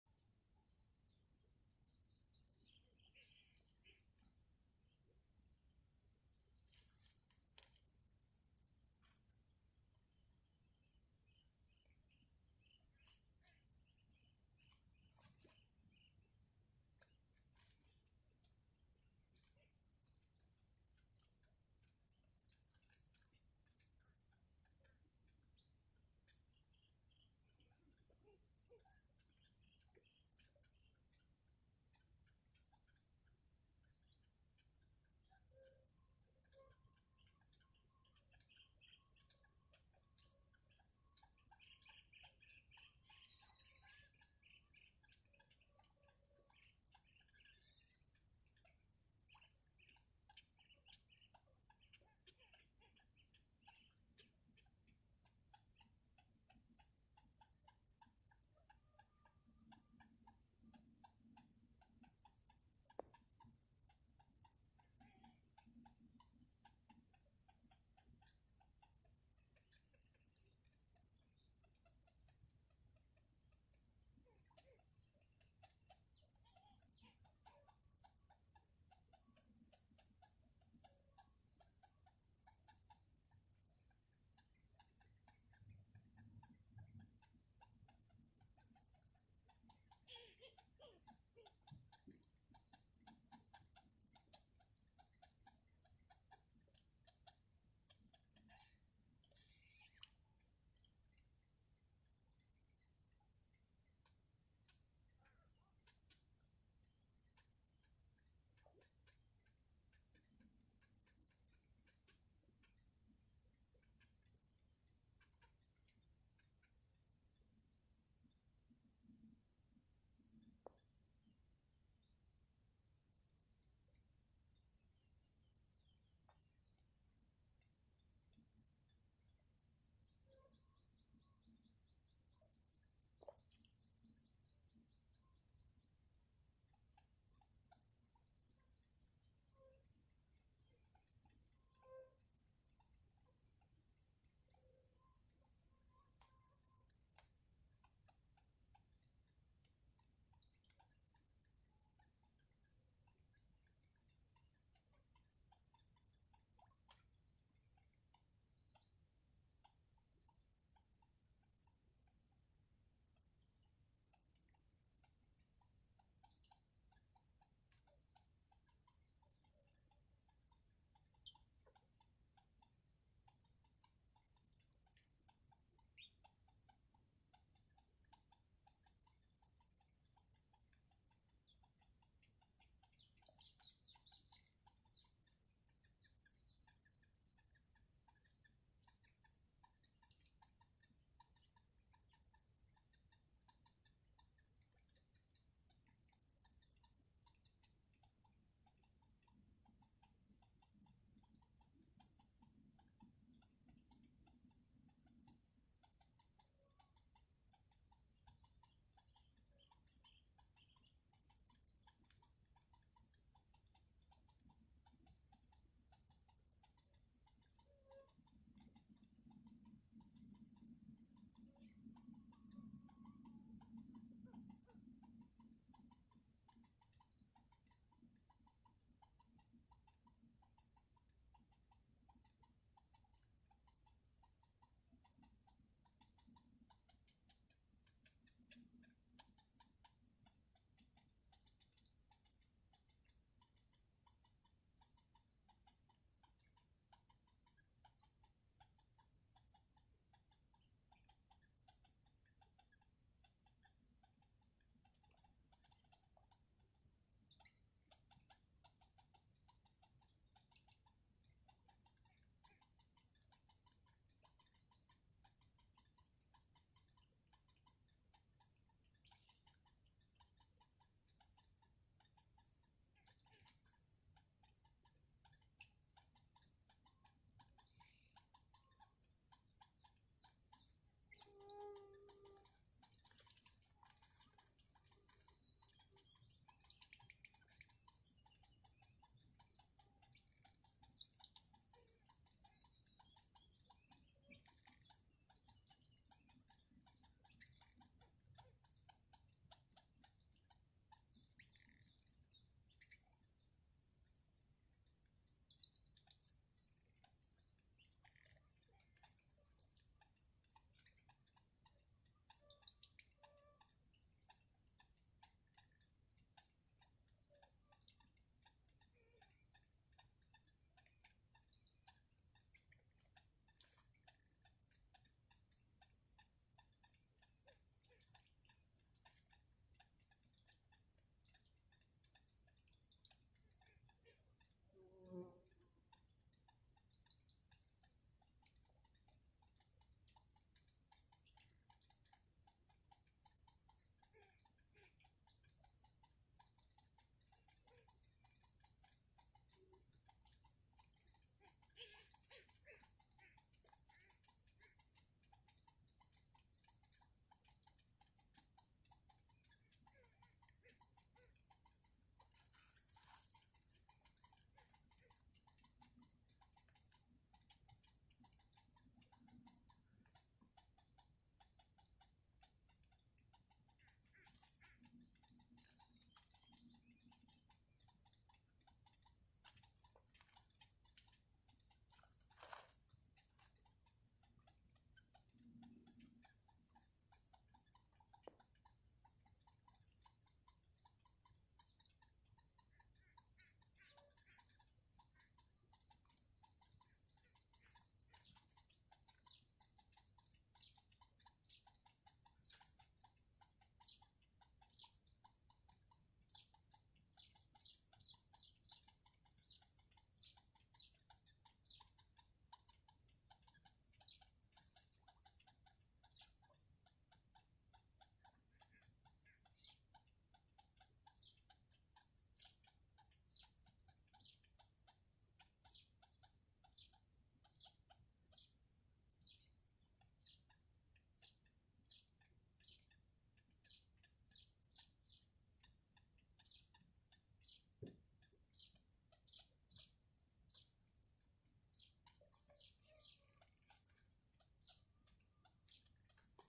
The Chicks Can Eat The Big Fish That The Mother Give Them
Bird Feeding Babies in The Nest, Amazing Mother Bird,